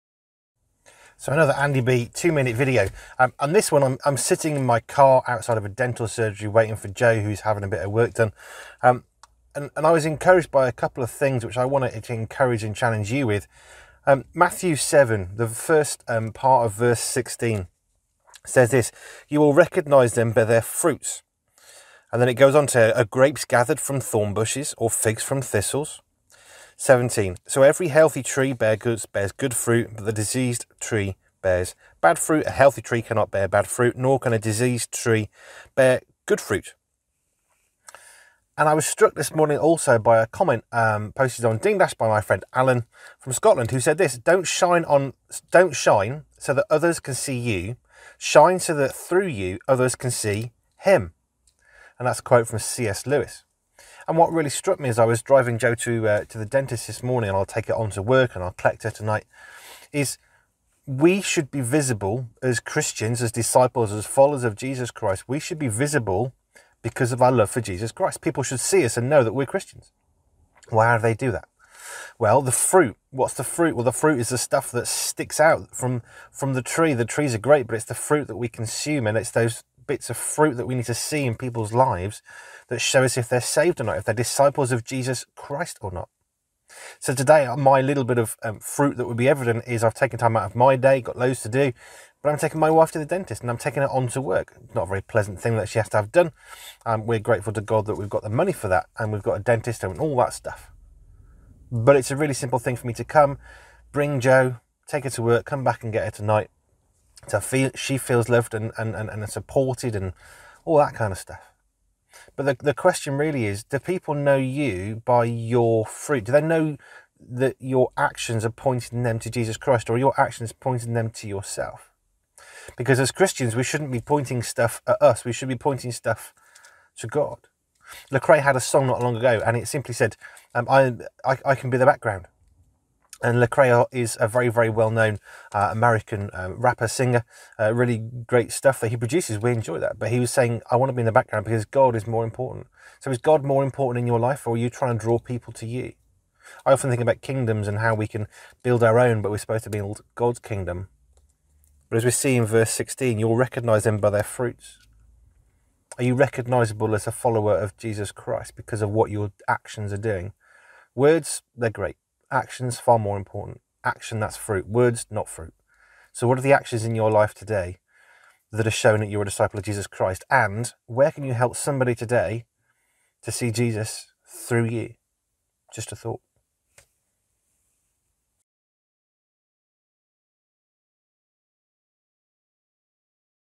Andy B 2 Minute Video, Fruit, Dentists, Words and Actions, S04E010
Matthew 7 verse 16 says that by our fruits we will be known.

This means that our actions are more important than our words because words, while very important, don’t necessarily lead to change, or action. But by their very definition, actions, are the outworking of what we truly believe and think.

Andy took his wife to the dentist and it made him think about what we can, each of us, do every single day to show the love of Jesus Christ to the people we meet, to the people we love, to people who know Jesus again, to people who need to know Jesus for the first time.

Every day is a learning day.

Every day is also an opportunity to serve Jesus.

We can choose either to move towards Jesus, or away from him.

So let’s make sure that, everyday, we are moving towards Jesus; that we are showing the love of Jesus to the people around us; that we are becoming more like Jesus Christ in all that we do and all that we say.

To quote CS Lewis “Don’t shine so that others can see you, shine so that through you, others can see him”.

So who can you encourage today?

Who can you text and say hello to?

Who can you email a photo to, from your walk, to brighten their day?

Who can you post a letter to reminding that Jesus loves them, even more than you do?

Let us be known by our actions, and may our actions match our words!

Andy B